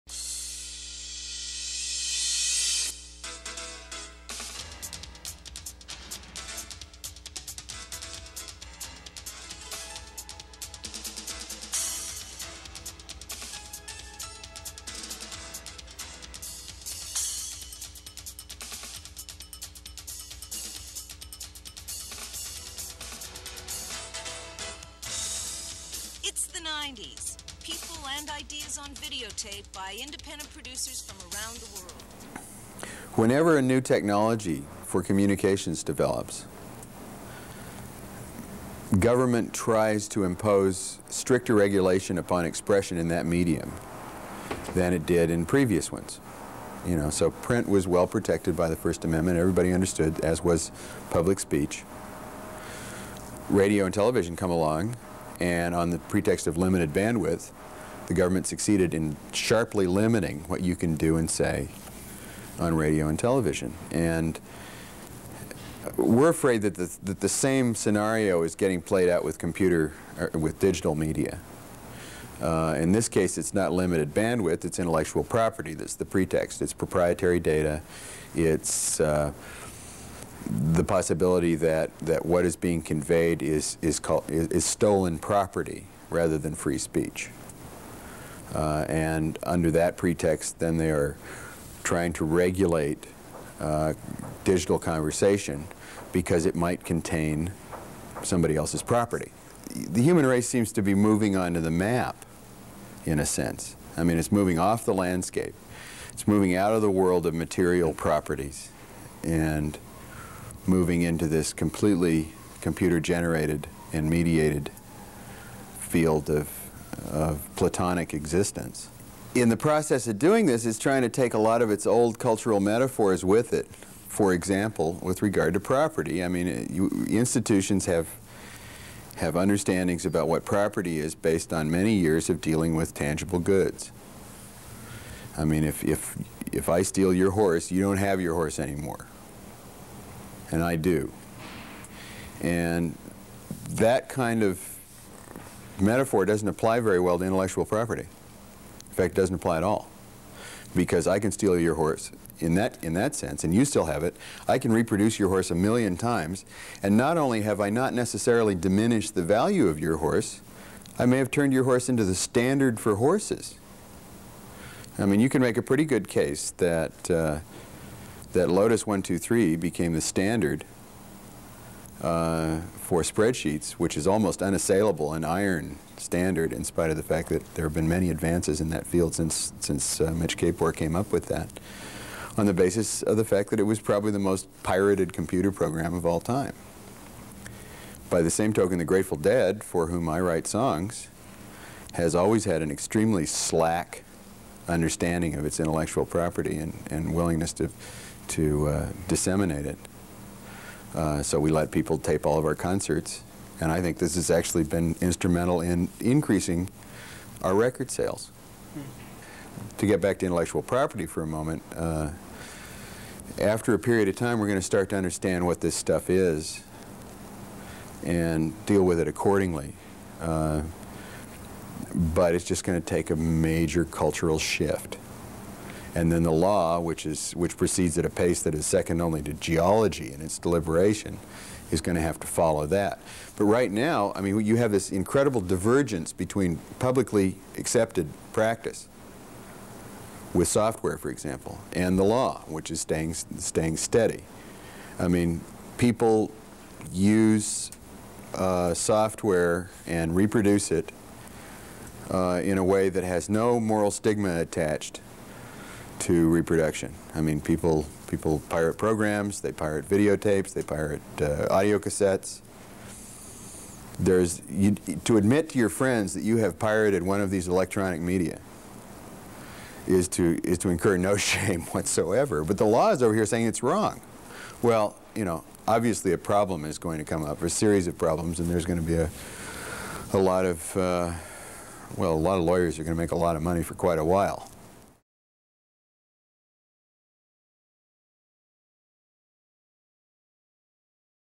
John Perry Barlow on Digital Rights
A 1991 interview with John Perry Barlow, Grateful Dead lyricist and founder of the Electronic Frontier Foundation, on issues surrounding intellectual property in the electronic era.